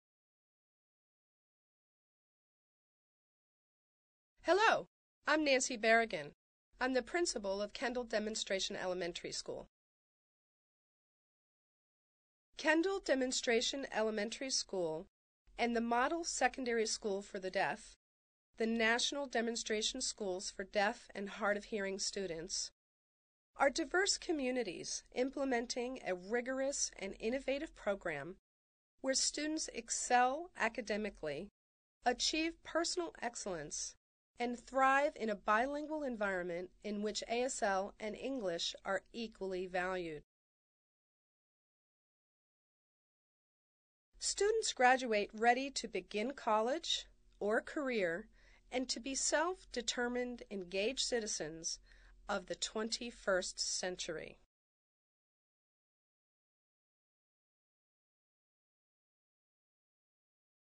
Mission Statement at Kendall Demonstration Elementary School - English
The Principal of the Kendall Demonstration Elementary School signs the Mission Statement.

This is the English version.